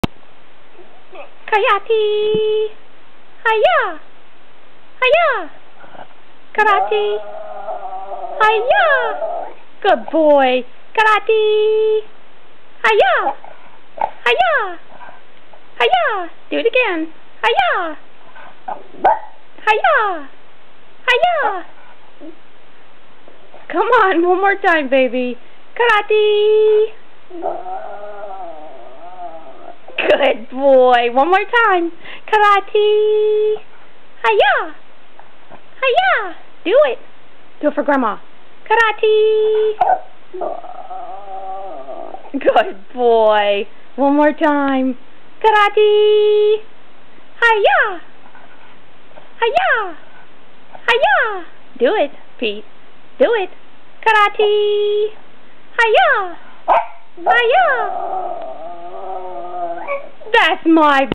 Peatie Howling
This video captures Peatie a few weeks before he started having seizures which have rendered him unable to howl any longer.